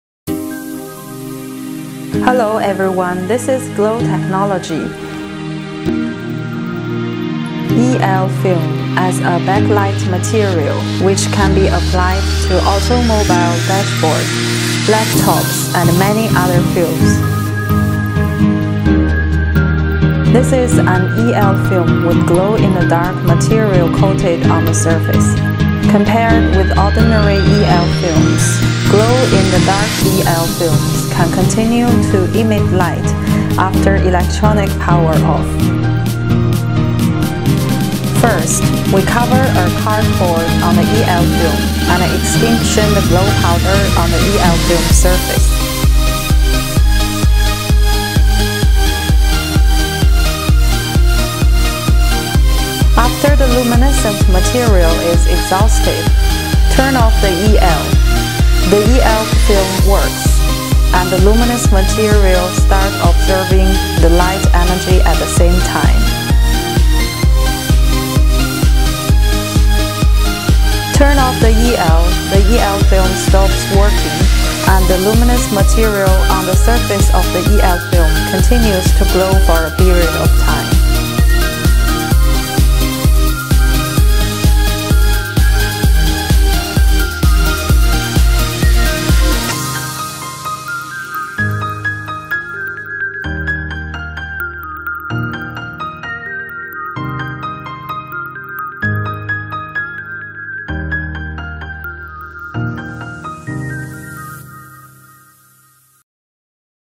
Glow in the dark +Electro Luminescent
EL film as a backlight material, which can be applied to automobile dashboards, laptops, and many other fields

This is an EL film with glow in the dark material coated on the surface. Compared with ordinary EL films, glow in the dark EL films can continue to emit light after electronic power off.

First, we cover a cardboard on the EL film and extinction the glow powder on the EL
film's surface

After the luminescent material is exhausted, turn on the EL, the EL film works, and the luminous material start absorbing the light energy at the same time

Turn off the EL, the EL film stops working, and the luminous material on the surface of the EL film continues to glow for a period of time